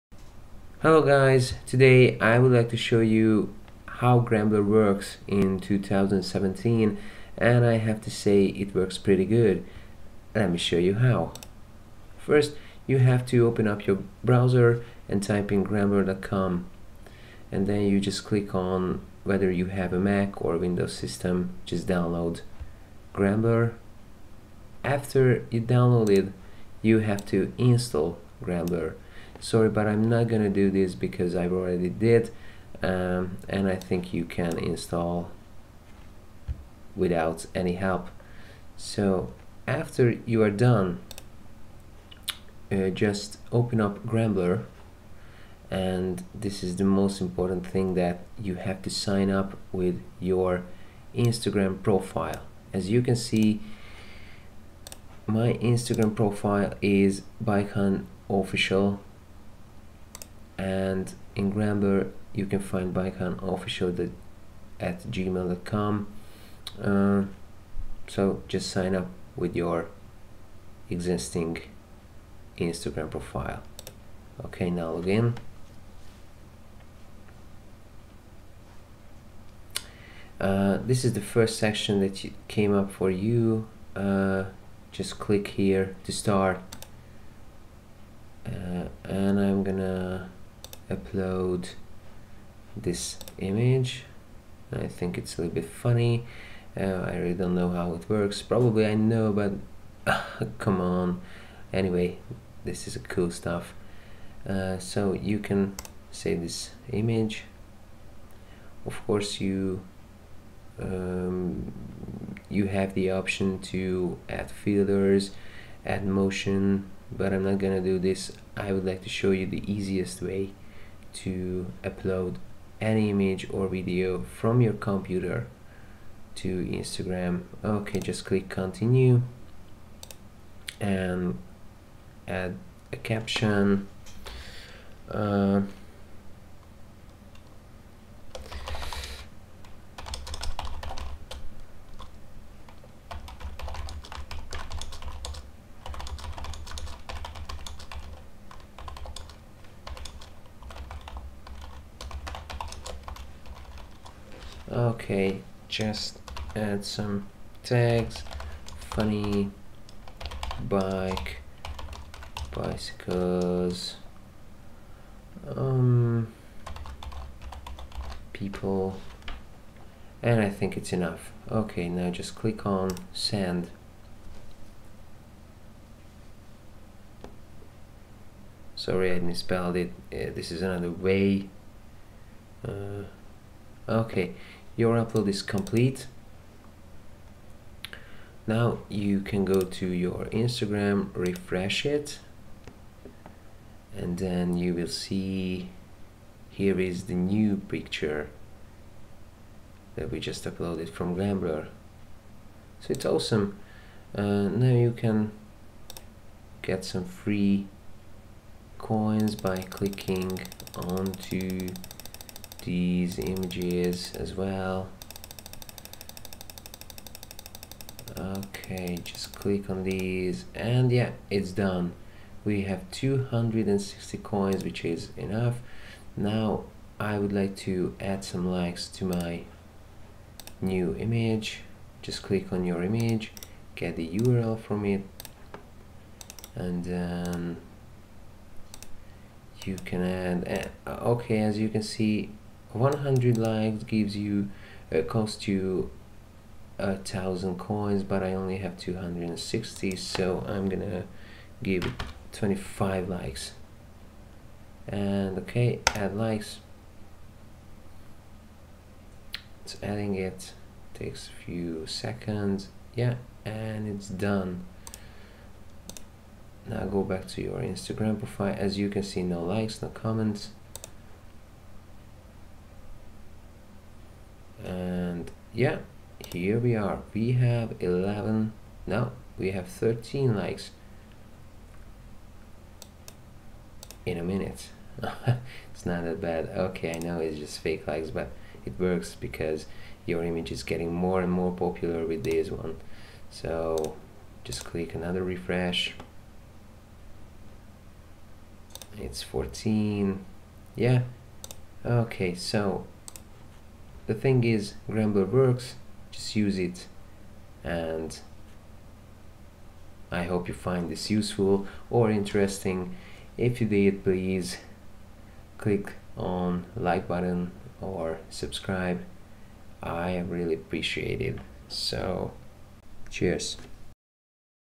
How to upload Photos & Videos on Instagram from Computer - Gramblr in 2017 +++ 100% WORKS +++
Hello Guys! Today, I would like to show you """HOW GRAMBLR WORKS IN 2017""" And...Yes, it works pretty good. You only have to do is download Gramblr -- Sing up with your Instagram Profile -- Start upload any content you want :)

-Bikehun